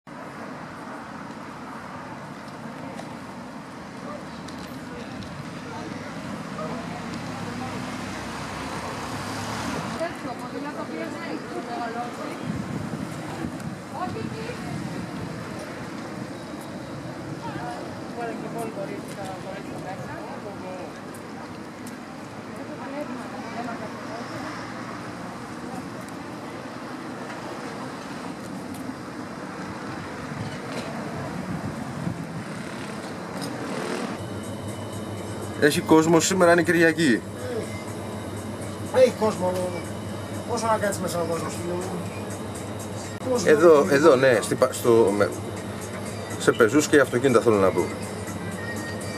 EviaZoom.gr: Πλήθος κόσμου στη παραλία της Χαλκίδας (10-01-2021)
Το αδιαχώρητο από κόσμο επικράτησε και στη παραλία της Χαλκίδας την Κυριακή (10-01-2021), με αφορμή την καλοκαιρία που επικράτησε...

Ο κόσμος ξεχύθηκε στο πεζόδρομο της παραλίας σαν να μην υπάρχει lockdown.

Στο EviaZoom.gr μίλησε και ο ιδιοκτήτης παραλιακού mini market της περιοχής, Νίκος Ζδράλης, ο οποίος είπε:

«Όντως έχει κόσμο, πόσο να κάτσει μέσα...»